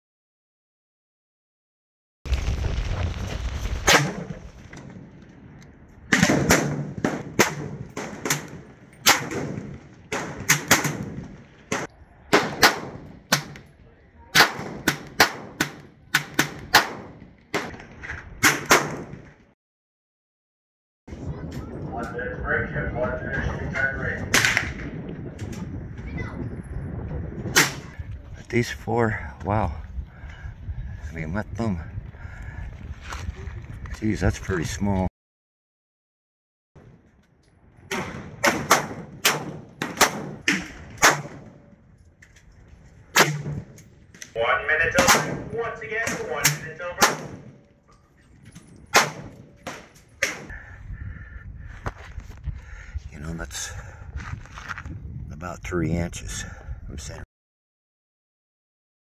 Bin-Tac S45 Sight-In & Challenger Pro .25 100 to 200 Yards
Quick video of the Bin-Tac S45 .457 Cal and the AEA Challenger Pro in .25 Cal...AGAIN the pro shooting long range with confidence!!!